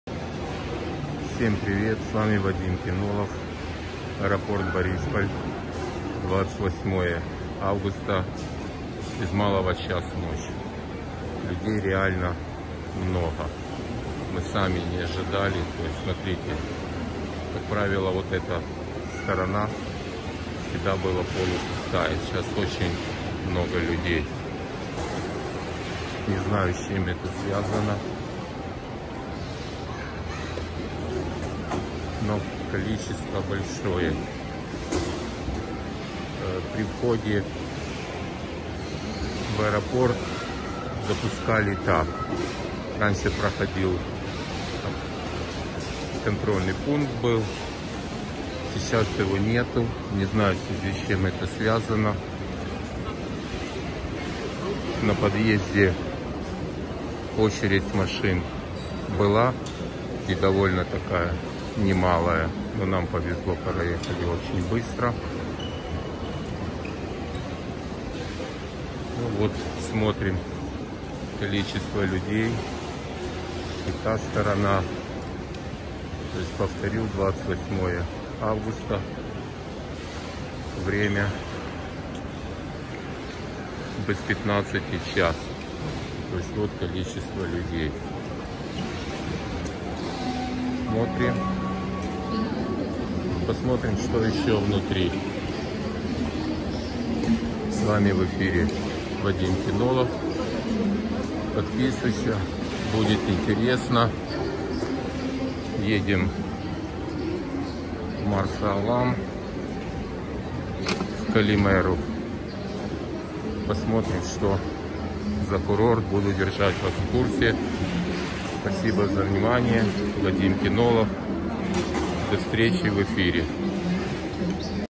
Свободных мест нет!!Аэропорт Борисполь 27.08.21. 0.45.
Людей кошмар как много!Аэропорт Борисполь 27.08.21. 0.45.
Все куда-то летят и я решил тоже!